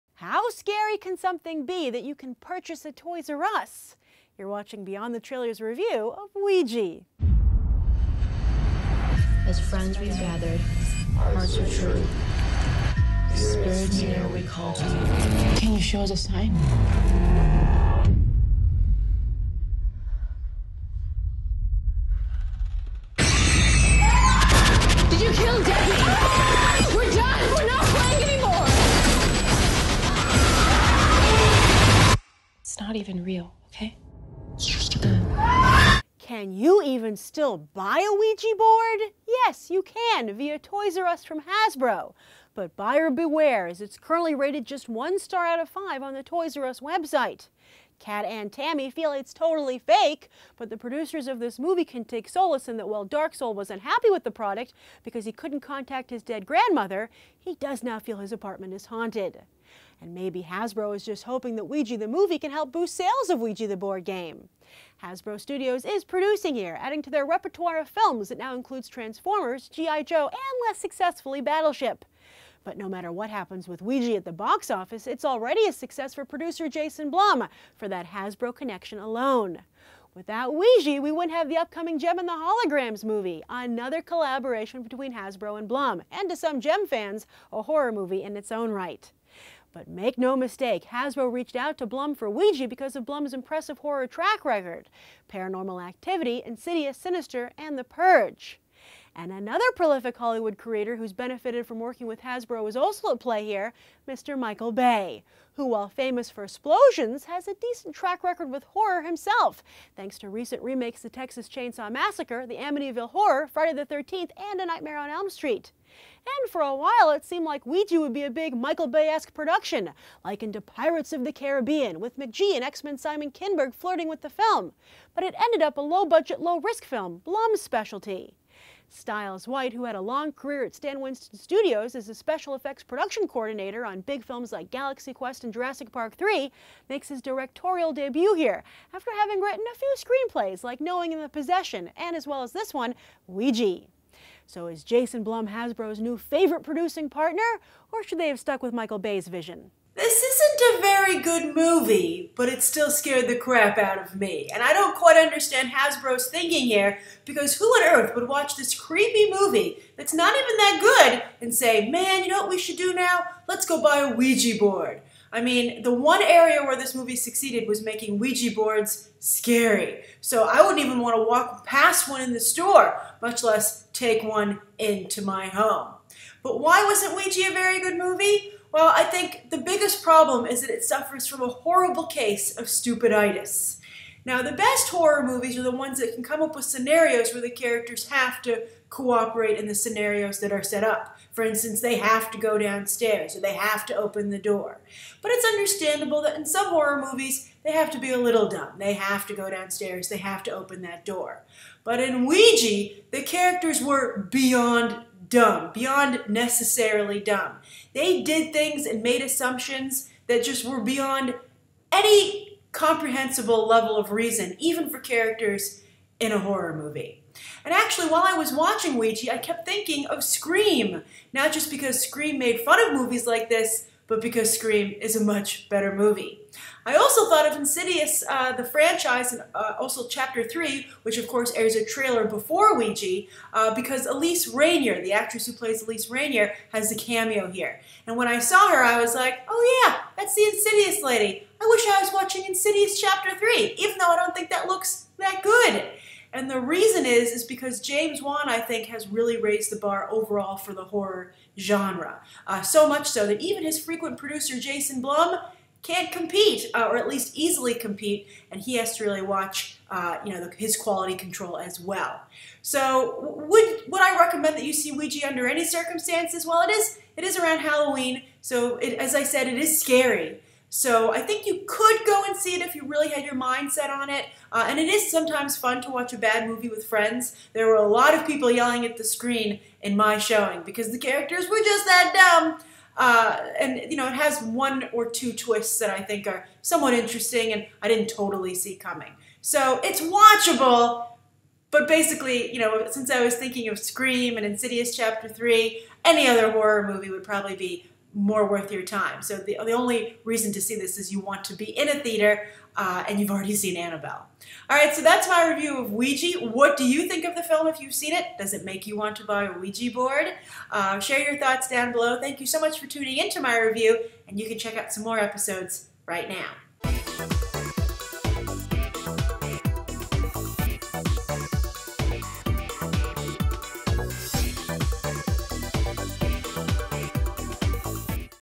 Ouija Movie Review - Beyond The Trailer
Ouija movie review!  Beyond The Trailer host Grace Randolph shares her review aka reaction today for this 2014 movie!

Ouija Movie Review.  Beyond The Trailer host Grace Randolph gives you her own review aka reaction to Ouija! Was Hasbro right to make a horror movie about its Ouija board game?! Should you see the full movie?  Enjoy Ouija in 2014, and make Beyond The Trailer your first stop for movie news, trailer and review on YouTube today!